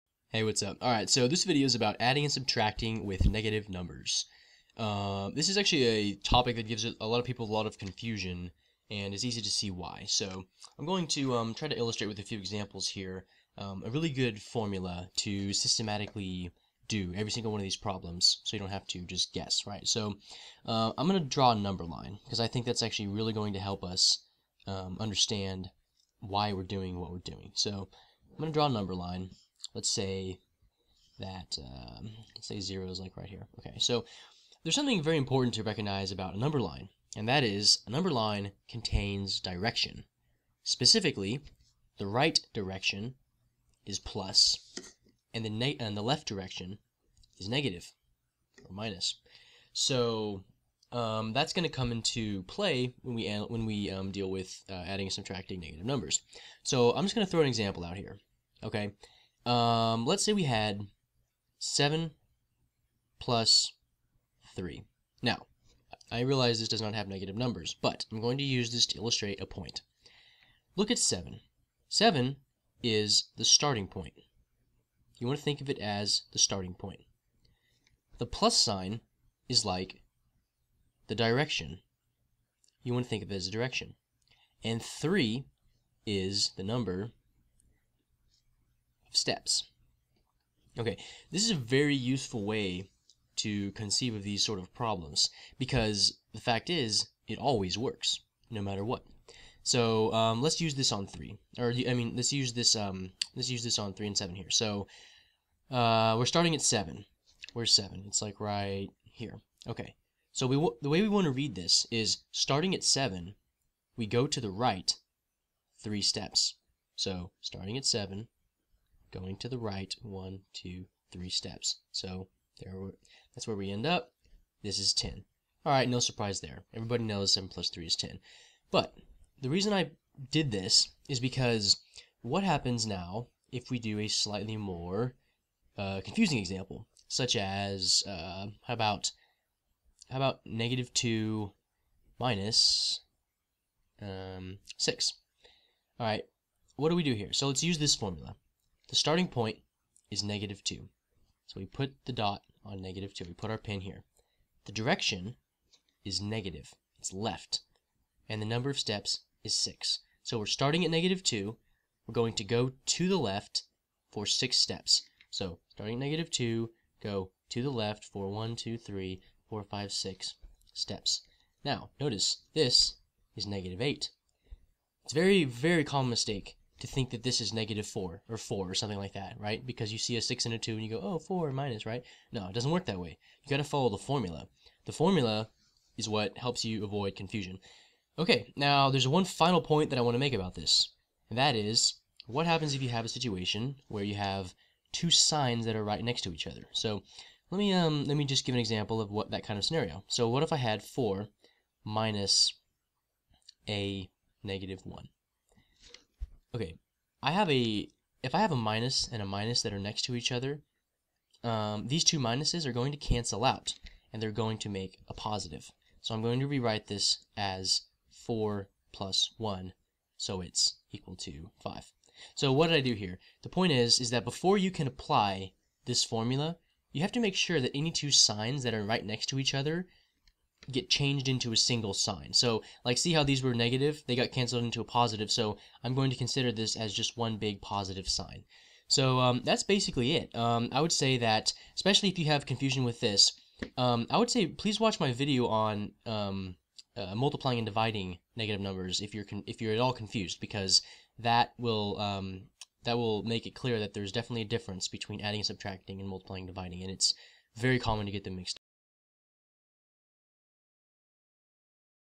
Adding and Subtracting Negative Numbers - Best Explanation
Explains how to add and subtract negative numbers based on conceptualizing the number line.